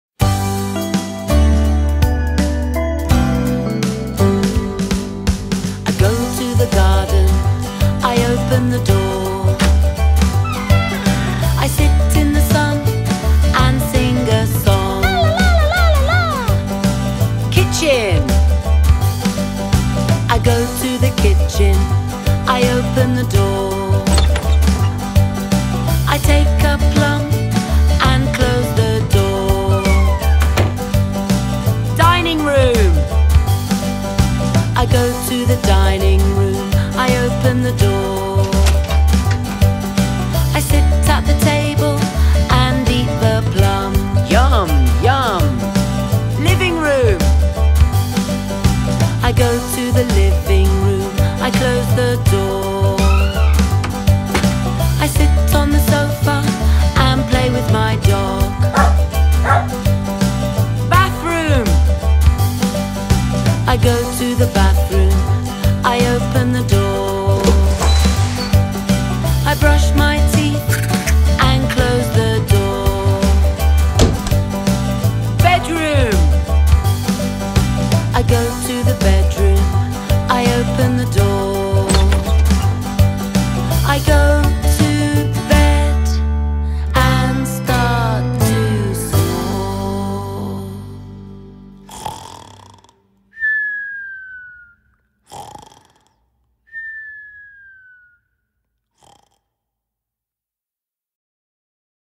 Inglés para niños con Greenman and the Magic Forest: Nivel B Unidad 2: I open the door
En este vídeo del nuevo curso de Educación Infantil de Cambridge University Press, Amy Jo lleva a los niños a visitar una casa para que repasen el vocabulario de esta unidad (bedroom, living room, kitchen, garden, dining room, bathroom) y los conceptos open/close.

Además, los niños también practican estructuras sencillas como I play with my dog  basadas en vocabulario aprendido en otras unidades. Las preciosas ilustraciones, las sencillas acciones y la repetición de estructuras ayudarán a los niños a relacionar palabras e imágenes y a reforzar el aprendizaje, ¡mientras se divierten y disfrutan!

Para más información sobre Greenman and the Magic Forest, y sobre todos nuestros cursos y recursos de inglés, visita: